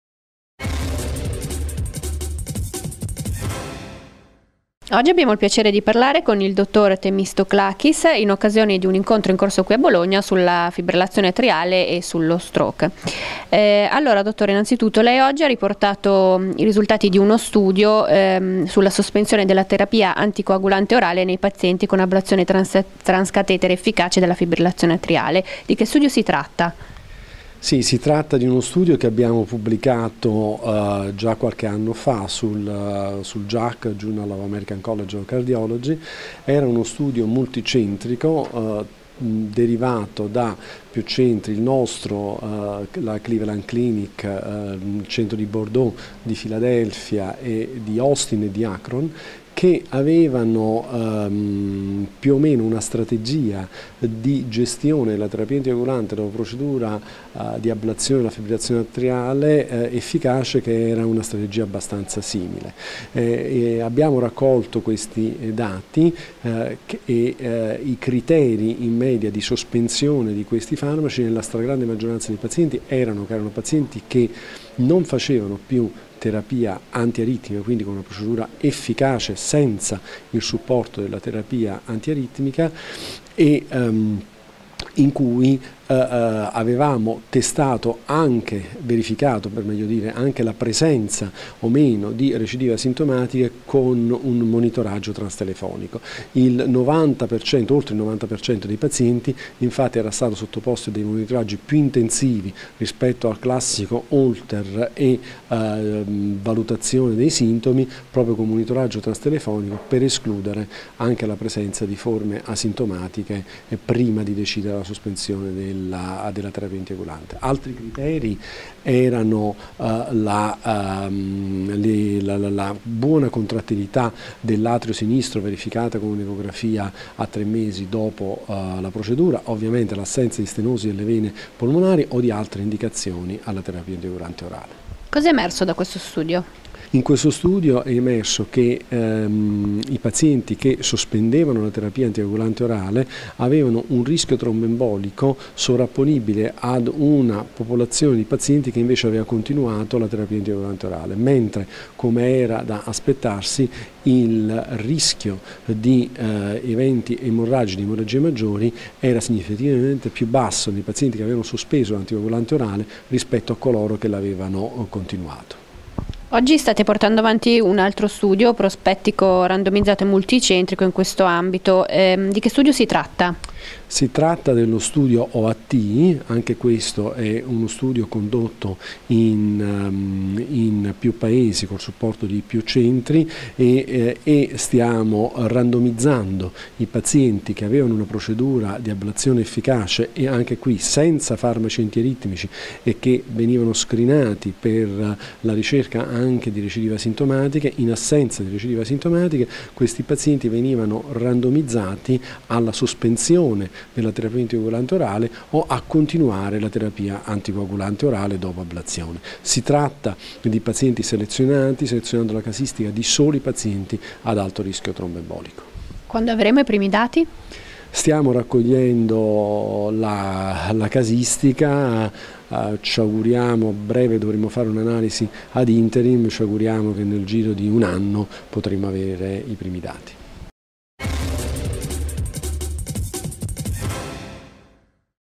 Interruzione della terapia anticoagulante orale dopo ablazione transcatetere della FA
Intervista al Dott. Sakis Themistoclakis, Responsabile del Centro per il trattamento della fibrillazione atriale, Ospedale dell'Angelo, Mestre.